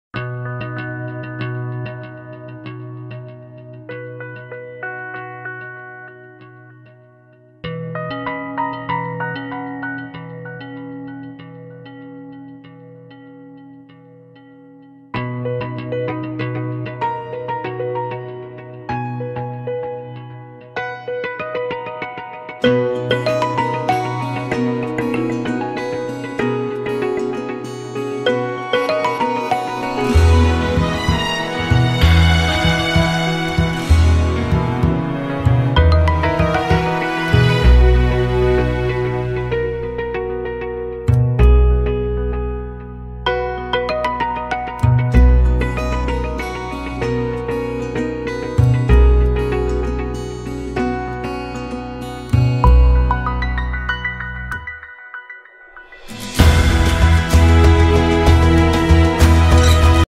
BABY HAMSTER EATING CORN
This video is just wayy to cute😊